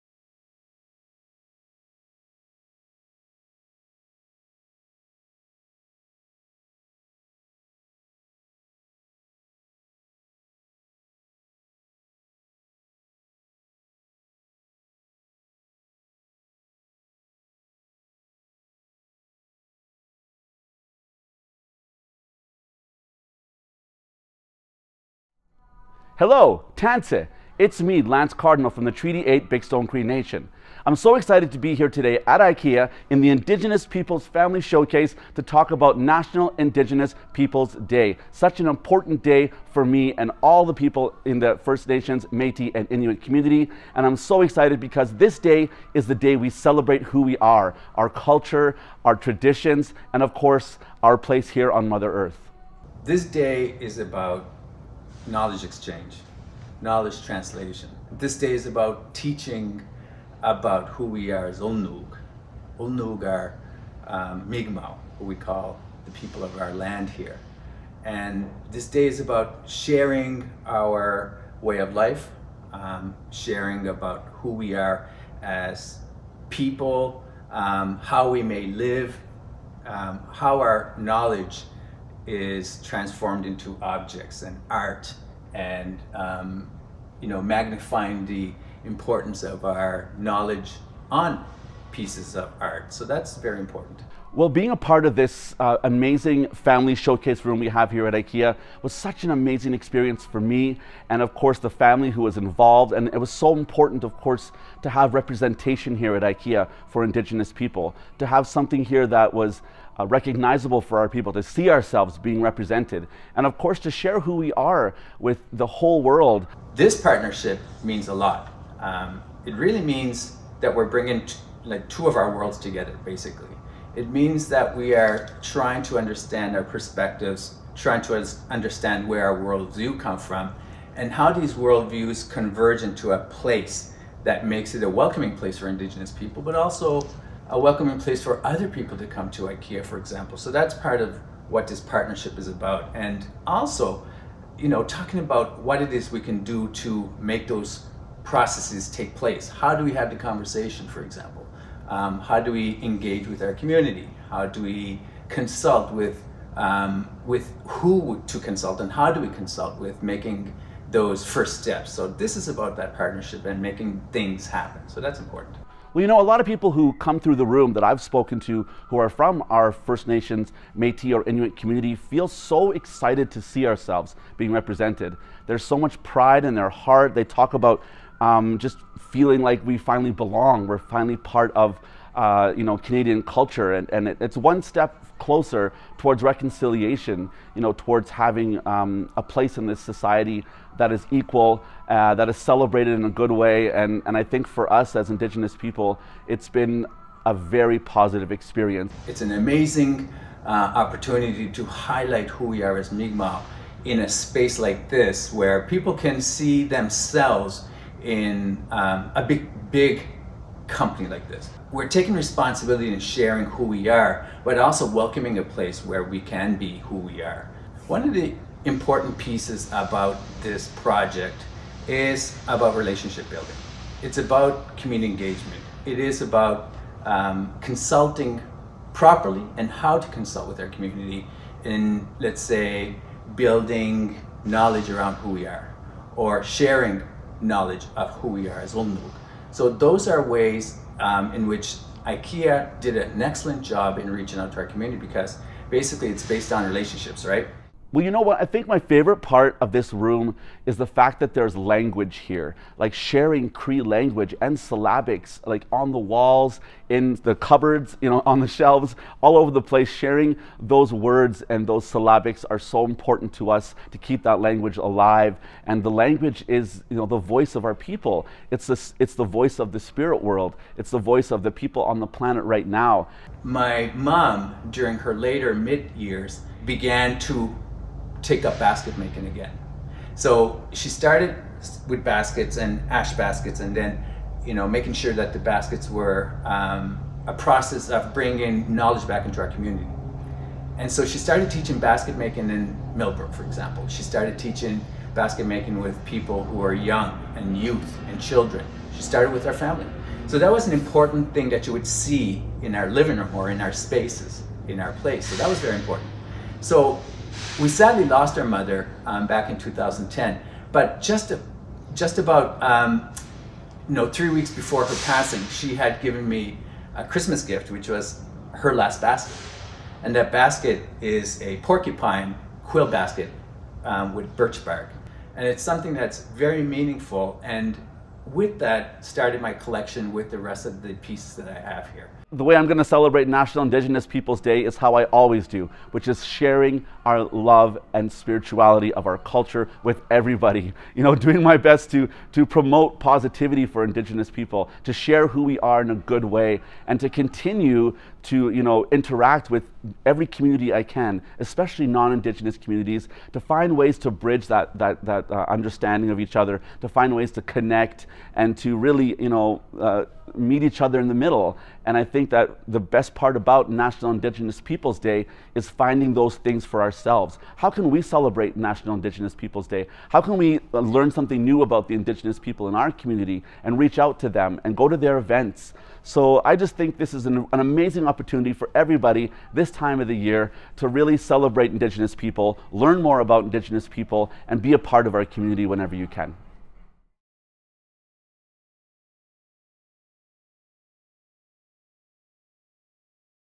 IKEA Canada | Les salles d’exposition des peuples autochtones, avec Lance Cardinal et John Sylliboy
Chez IKEA, nous nous soucions des peuples autochtones et sommes résolus à faire résonner et à amplifier leurs voix. Ces derniers mois, nous avons eu l’honneur de collaborer avec l’artiste cri Lance Cardinal et l’éducateur mi’kmaq John Sylliboy pour présenter le quotidien des personnes autochtones d’aujourd’hui dans nos toutes premières salles d’exposition autochtones. Dans cette vidéo, Lance et John expliquent comment ces salles ont vu le jour aux magasins IKEA d’Edmonton et d’Halifax.

Pour en savoir plus sur les engagements de IKEA en matière de diversité, d’égalité et d’inclusion, visitez ce